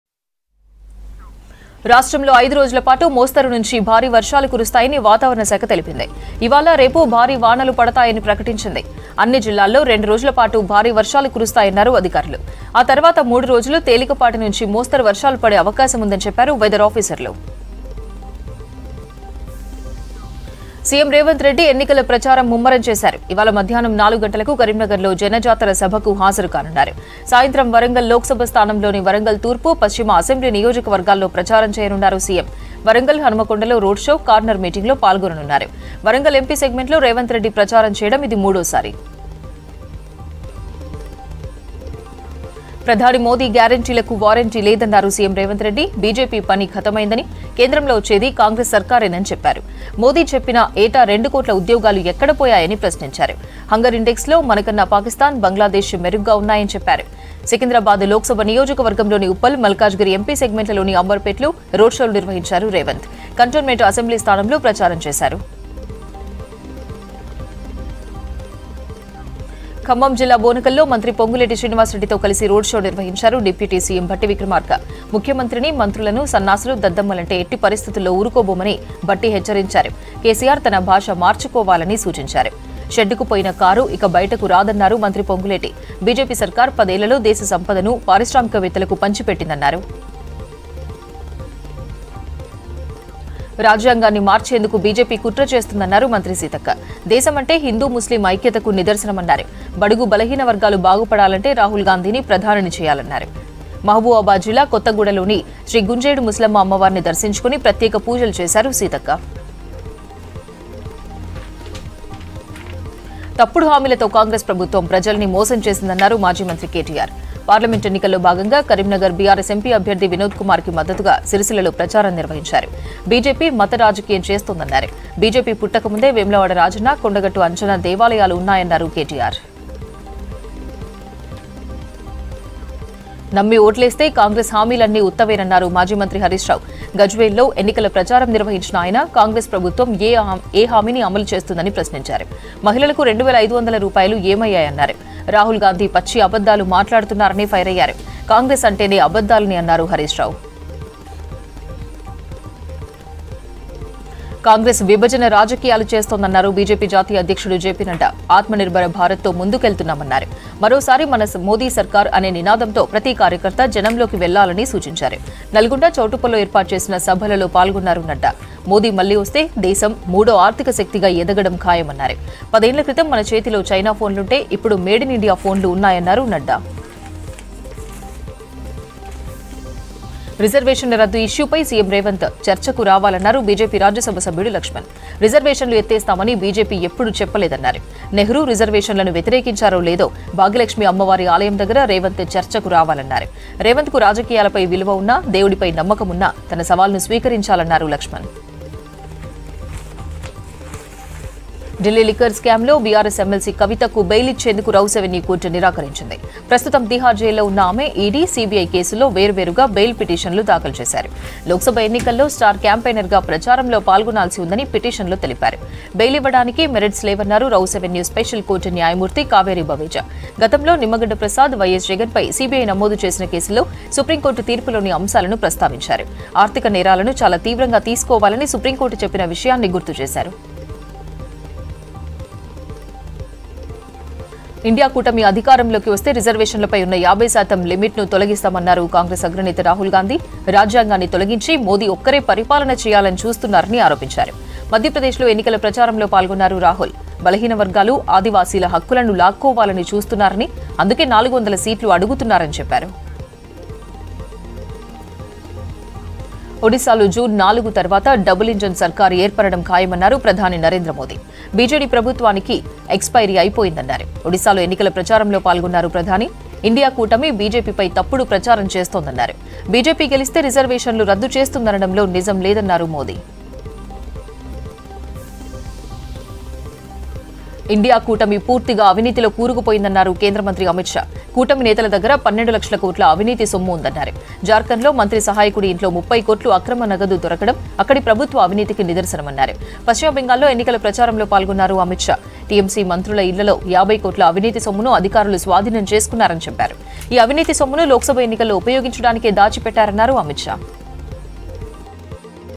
Heavy Rains In State For 5 Days | CM Revanth Is Busy With Three Meeting Today| KTR Campaign|Top News
Heavy Rains In State For 5 Days | CM Revanth Is Busy With Three Meeting Today| KCR Campaign|Top News